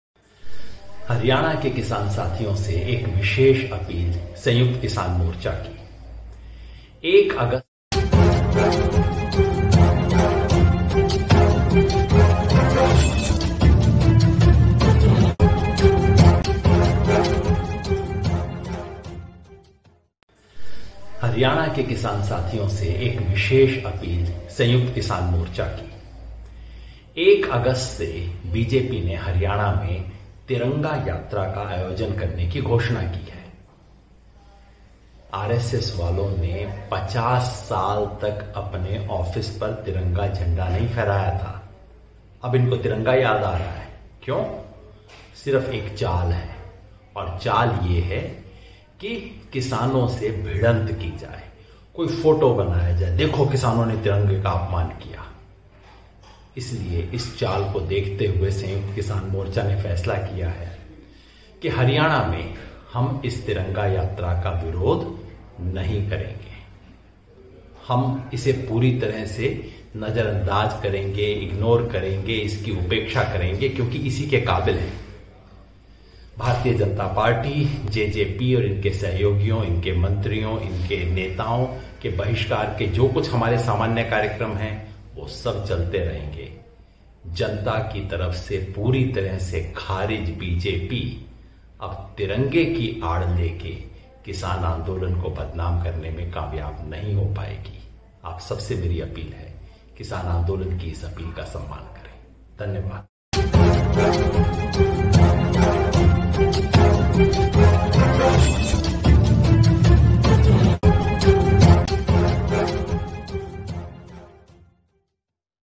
भाजपा तिरंगा यात्रा का विरोध नहीं करेंगे किसान ! क्या बोले, योगिंद्र यादव।
बीजेपी के द्वारा तिरंगा का विरोध नहीं करेंगे किसान, चाल में नहीं फसेंगे किसान _योगेंद्र यादव
योगेंद्र यादव ने वीडियो जारी करते हुए किसानों से अपील की है

kisanAndolan
manoharlal